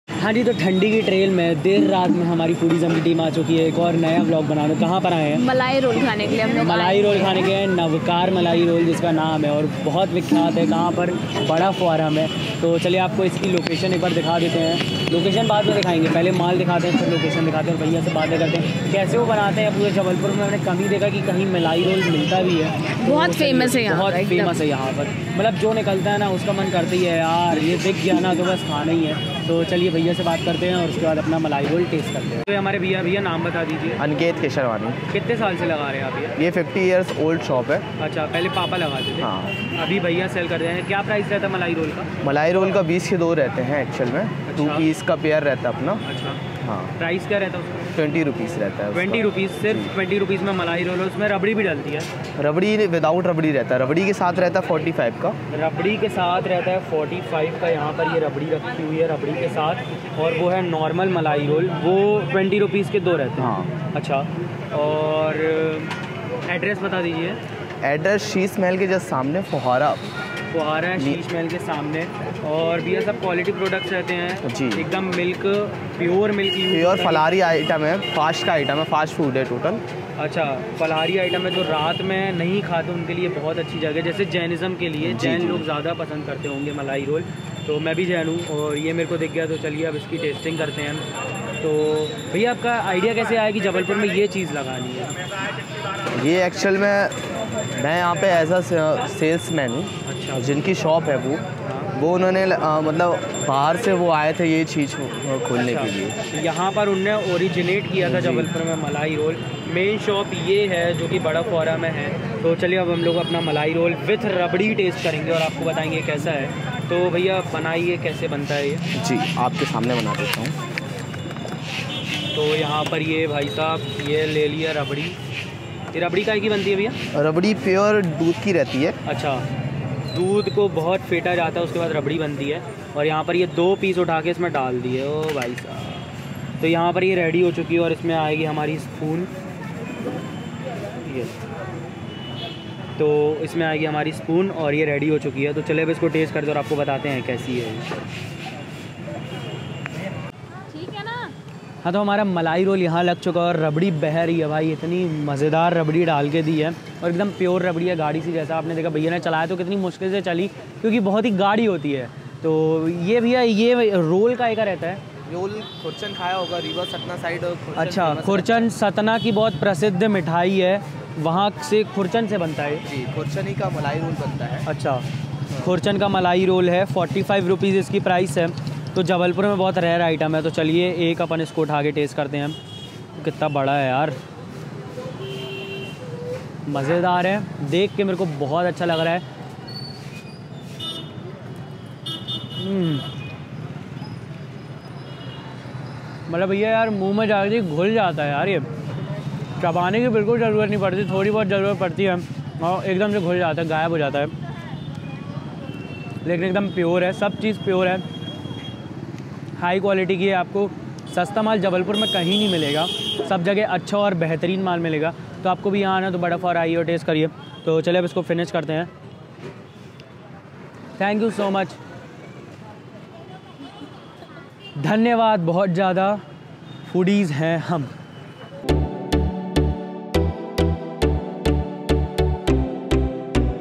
Famous Sweet of Bada Fuhara Malai Roll Rs.20/- Only I Indian Street Food I Jabalpur Fuhara Market
Social Media Platforms @FOODIE'S HAI HUMM